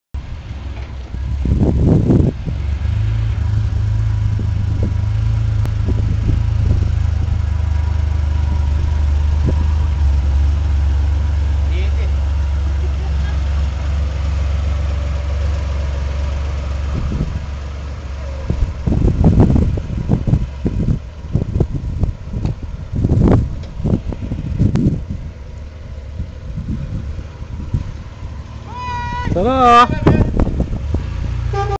Willys pick up
Keith and Penny out for a drive in there 41 willy's pick up, sorry about the sound ans skippy camera !